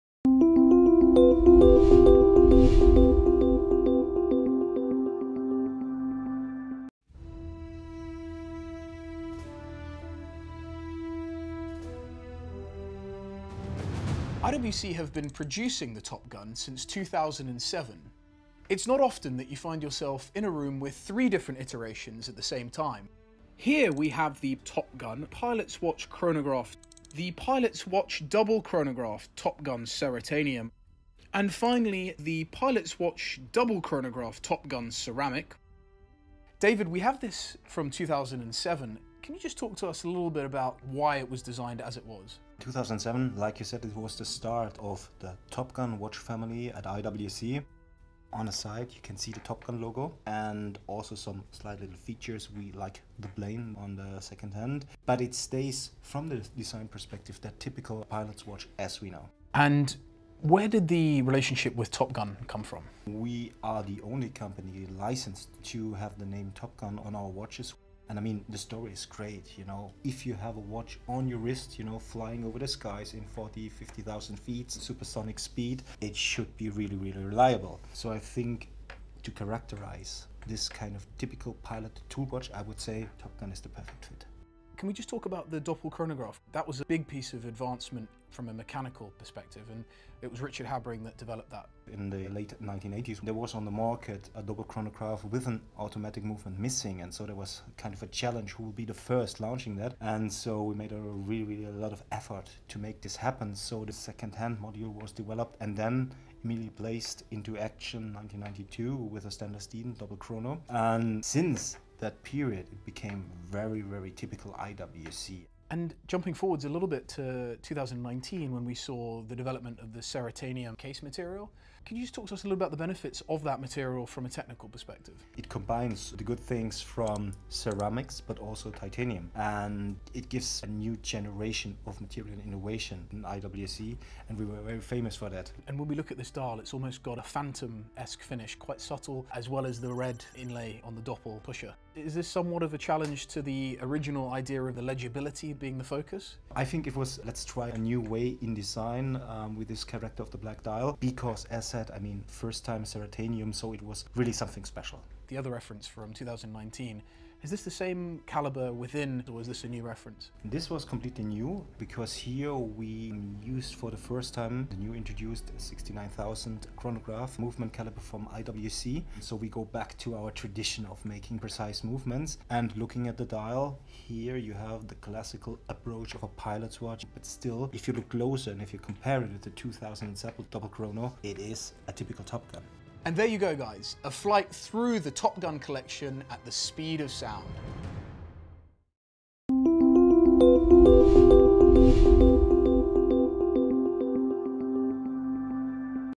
IWCHERITAGE: The Heritage of IWC Top Gun Pilot's Watches l Jura Watches
David and Justin discuss three IWC timepieces from the TOP GUN collection, Pilot’s Watch Chronograph, Pilot’s Watch Double Chronograph TOP GUN Ceratanium, Pilot’s Watch Double Chrono TOP GUN Ceramic. Discover IWC watches online and instore at Jura Watches!